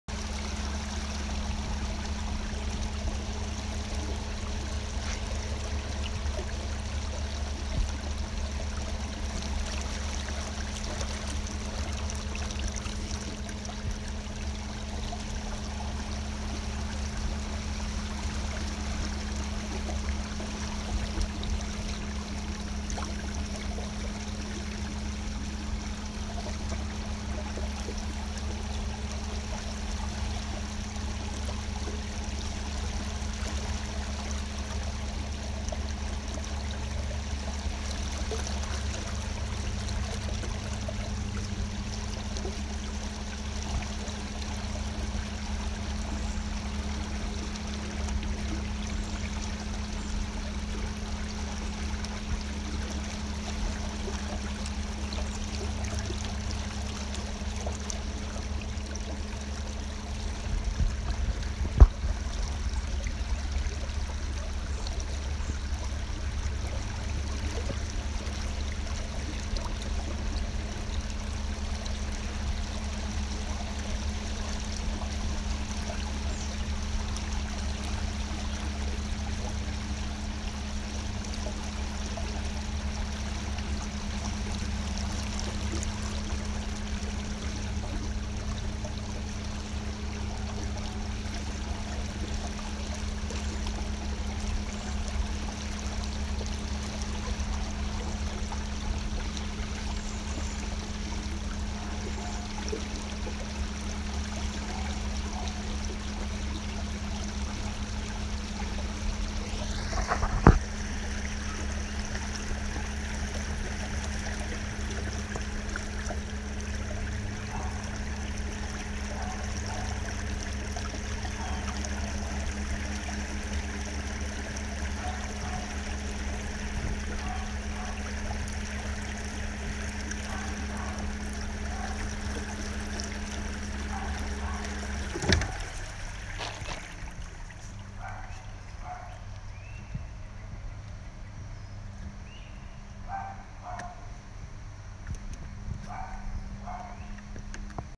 Waterfront home, Plaisance, Quebec, Canada
View of our neighbourhood from the river.
Notre voisinage vu à partir de la rivière.